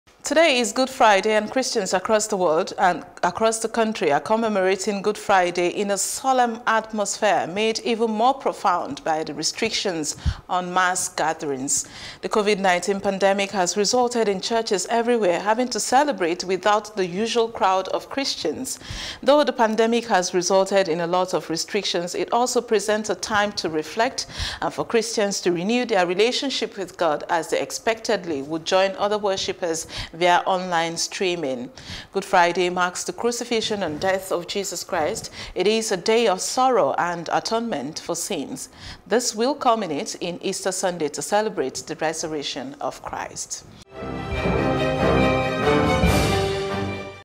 Christians Celebrate Good Friday In Solemn Manner
Today is Good Friday and Christians across the country are commemorating Good Friday in a solemn atmosphere made even more profound by the restrictions on mass gatherings.